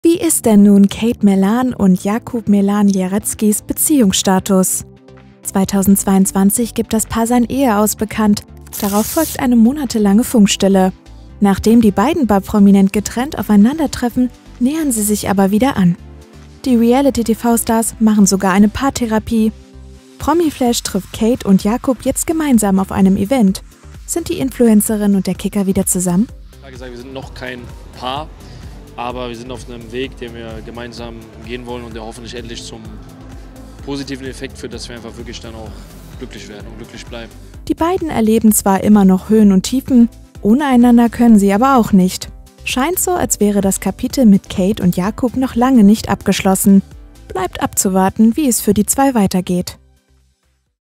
Gemeinsamer Auftritt: Sind Kate Merlan und Jakub ein Paar?
Kate Merlan (36) und Jakub Merlan-Jarecki (27) machen es spannend! Die Reality-TV-Bekanntheit und der Fußballer näherten sich bei Prominent getrennt wieder an, nachdem sie ihre Beziehung im vergangenen Jahr eigentlich beendet hatten. Daraufhin entschieden sich die beiden TV-Stars dafür, gemeinsam eine Therapie zu machen, um ihre Ehe zu retten. Nun erschienen sie gemeinsam beim Launch des TK-Maxx-Onlineshops in Köln. Ist das etwa ihr offizielles Liebes-Comeback? Kate und Jakub haben Promiflash über ihren Beziehungsstatus aufgeklärt... (awe/ega)